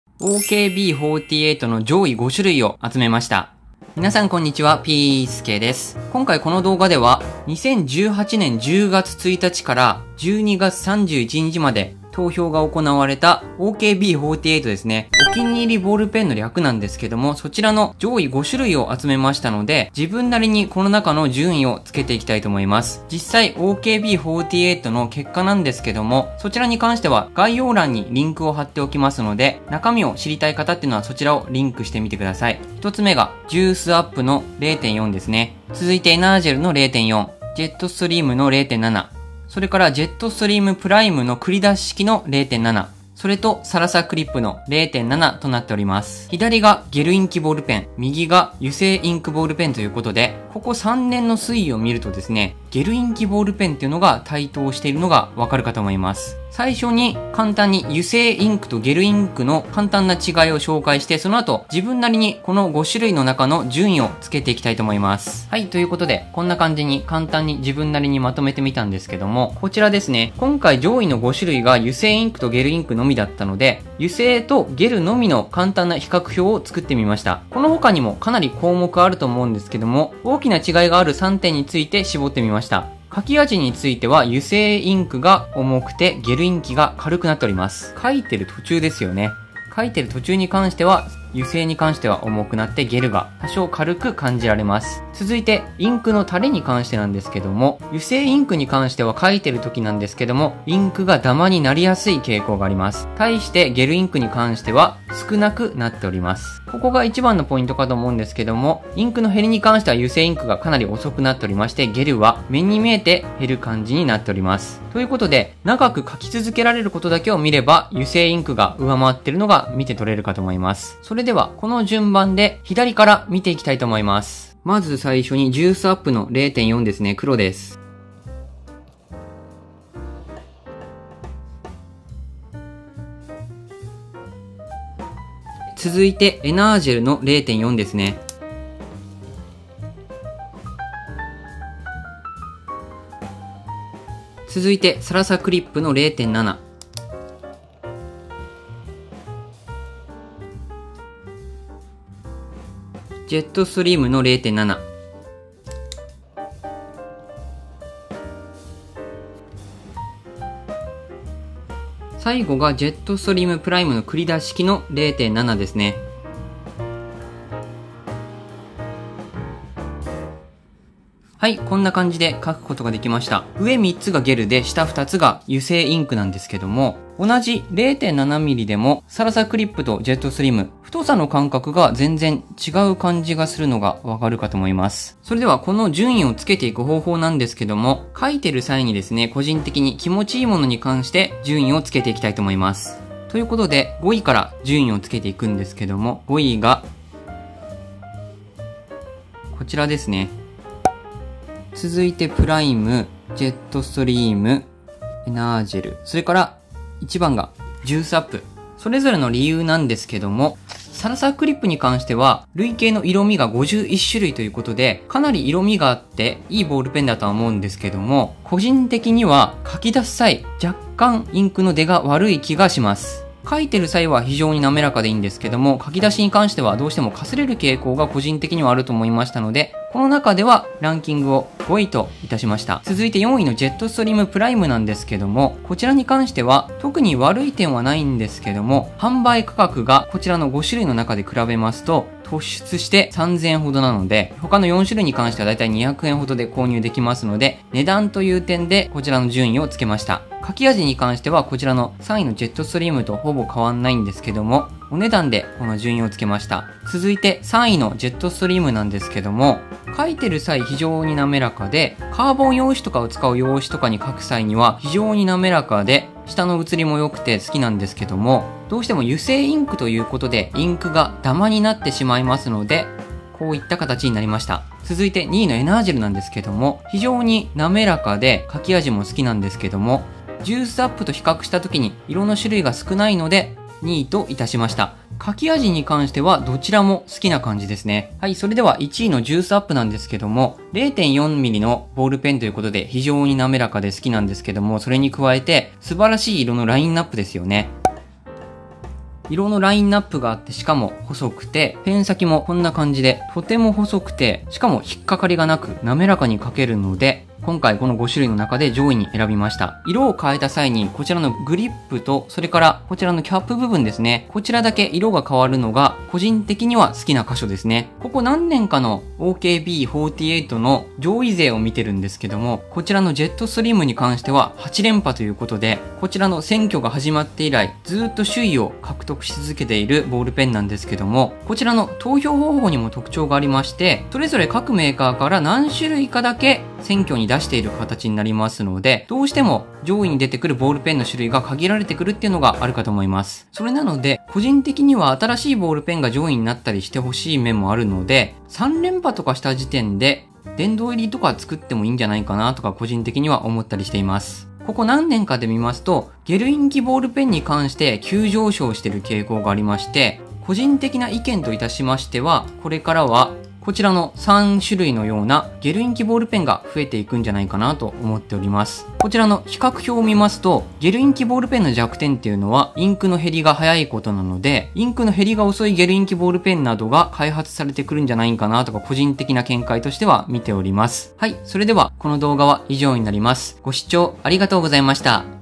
【OKB48】第8回 お気に入り ボールペン トップ 5 自分なりに変更
ぴーすけちゃんねる[pisuke_channel]へようこそ。
このチャンネルでは、日常生活での購入品を
紹介・レビューしていきます。

こんにちは♪動画をご覧いただきありがとうございます。
第8回お気に入りボールペン選抜総選挙
の結果に基づいて自分の好きな順番変える動画です。

各社いくつかのモデルを選挙に出馬させ、
人気投票により上位モデルを決める選挙です。
ジェットストリームが8冠を達成したということで
殿堂入りになっても良いのではないかと個人的には